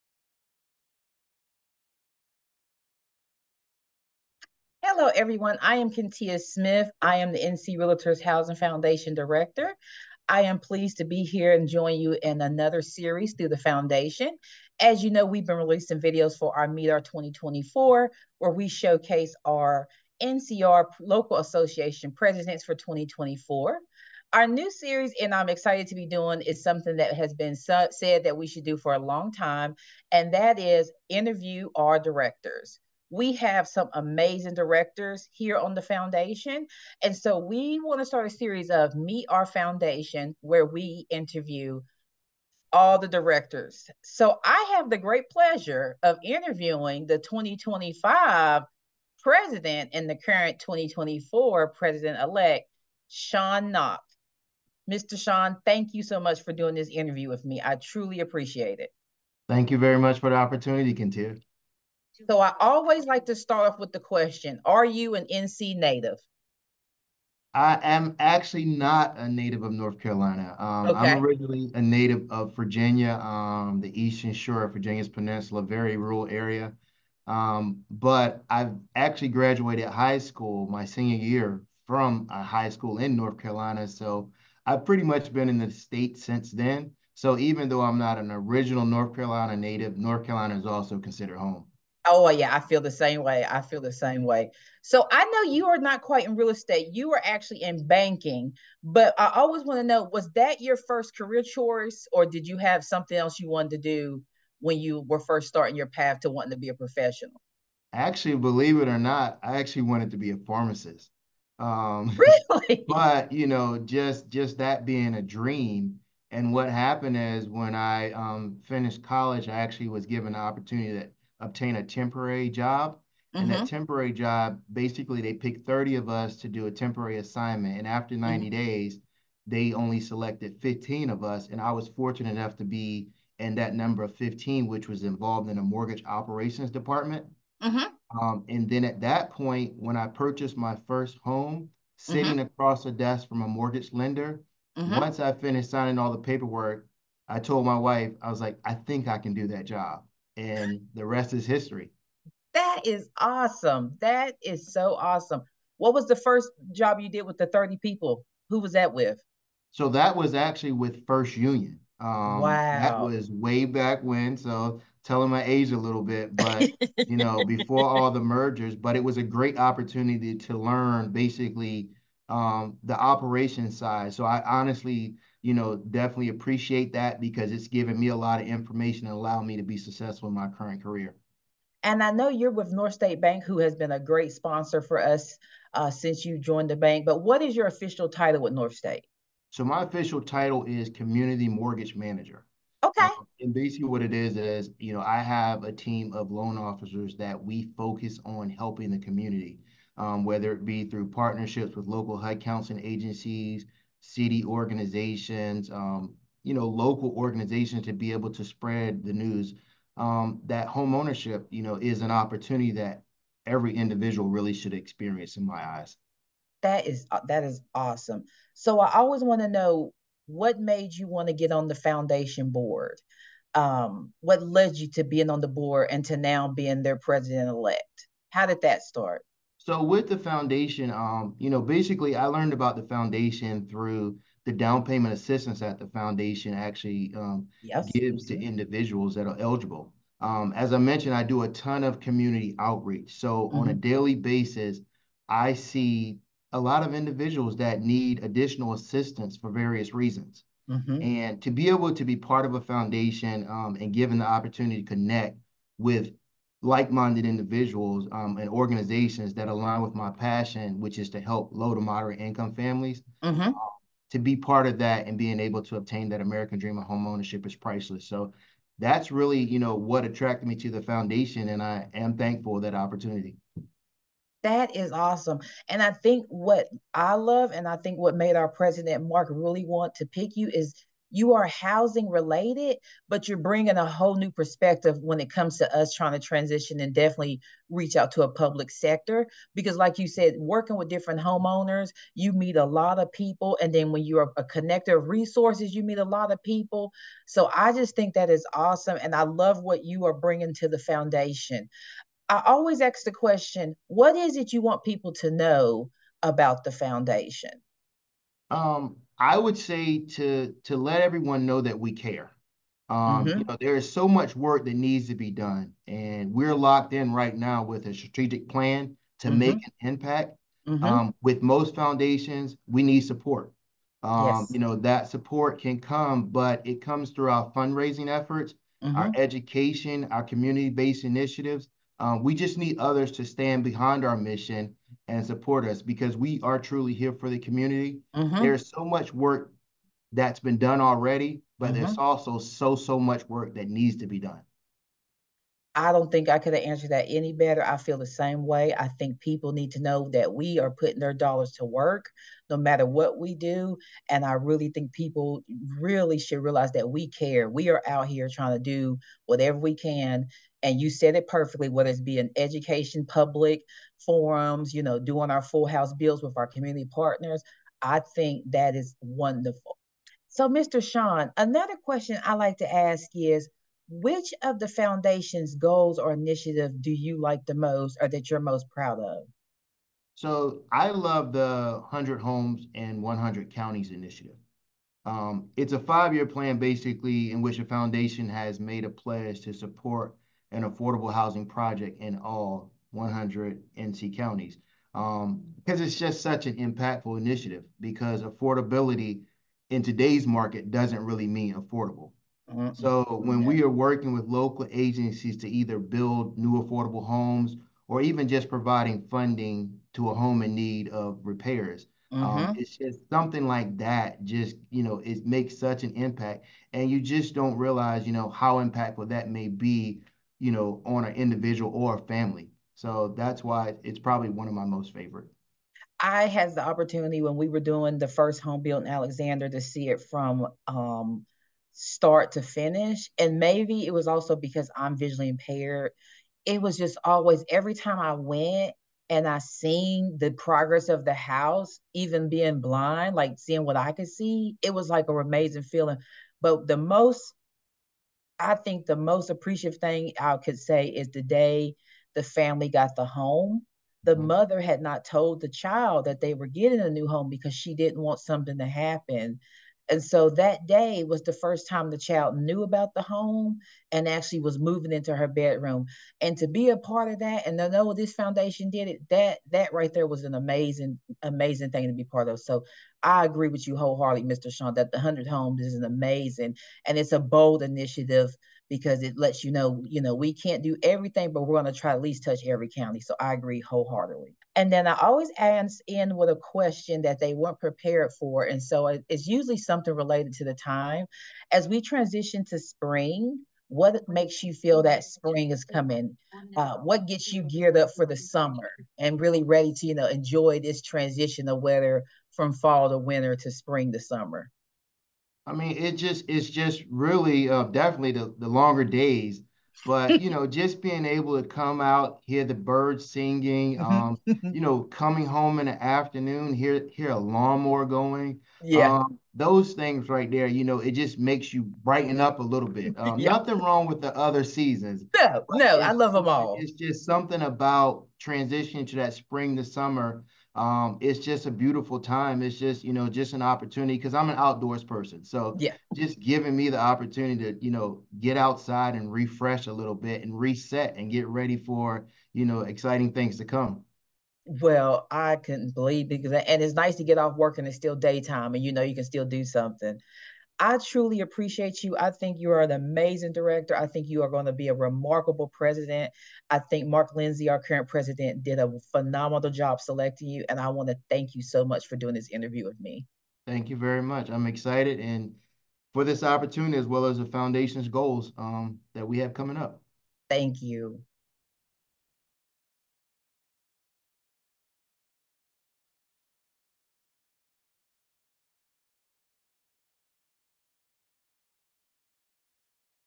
Meet Our Foundation: Interview with Sean Nock
Kentia Smith interviews incoming NC REALTORS® Housing Foundation President Sean Nock.